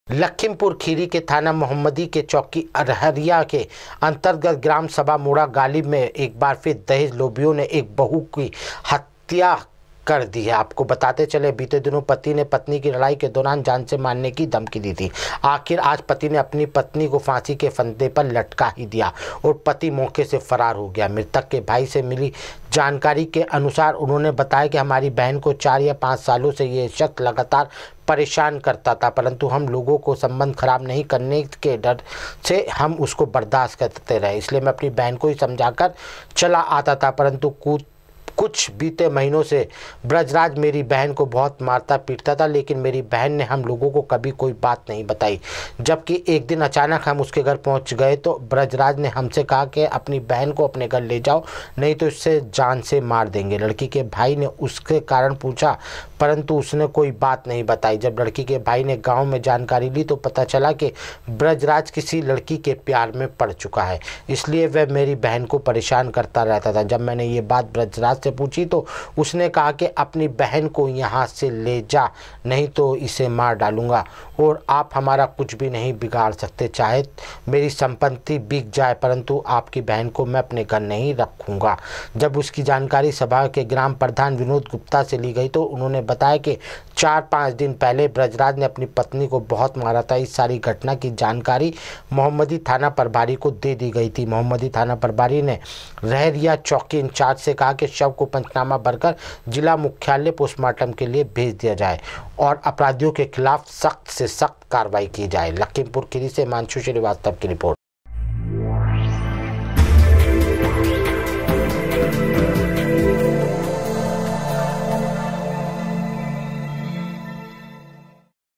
[ Lakhimpur ] लखीमपुर खीरी में दहेज लोभीयों ने की बहू की हत्या / THE NEWS INDIA
लखीमपुर खीरी के थाना मोहम्मदी की चौकी रहरिया के अंतर्गत ग्राम सभा  मोड़ा गालिब  में फिर दहेज लोभी ने की बहू की हत्या आपको बताते चलें बीते दिनों में पति ने पत्नी को लड़ाई के दौरान जान से मारने की दी थी धमकी आखिर आज पति ने अपनी पत्नी को फांसी के फंदे पर लटका दिया